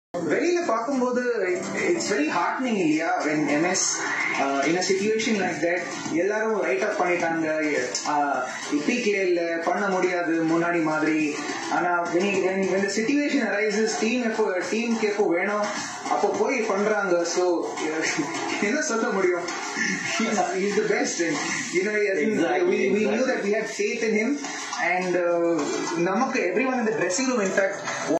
Man Spoke my Heart out! 💖| Uthappa Spoke about MS Dhoni | Comeback dhoni | Vintage Dhoni | AK Editz
DC VS CSK MATCH || CSK WINNING WHATSAPP STATUS TAMIL || CSK WINNING STATUS || CSK GOES TO FINAL
Match
The IPL 2021 match between DC and CSK
streaming live on Star Sports channels and also in Dinesy+Hotstar.

CHENNAI SUPER KINGS WON THE MATCH AND QUALIFIED FOR IPL 2021 FINALS

Overview:
A solid 83-run partnership between Rishabh Pant (51) and Shimron Hetmyer (37) took DC to 172  runs against Chennai Super Kings in Qualifier 1. Prithvi Shaw also played a blazing 34-ball 60 knock for Delhi Capitals.